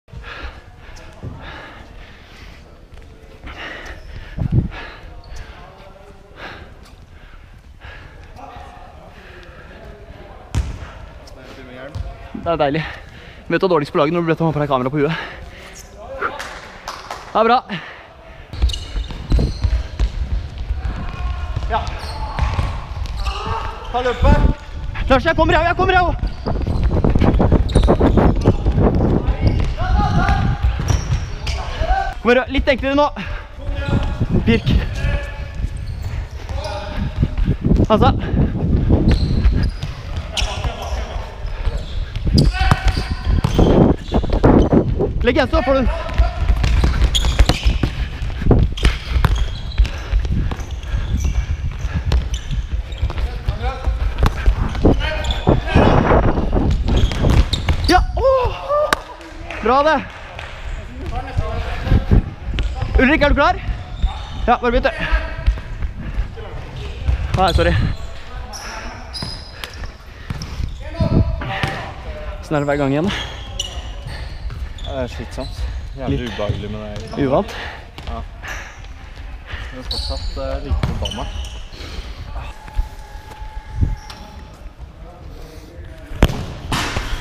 Futsal-trening POV
Få et lite innblikk i hvordan en trening med Lillehammer Futsal kan se ut.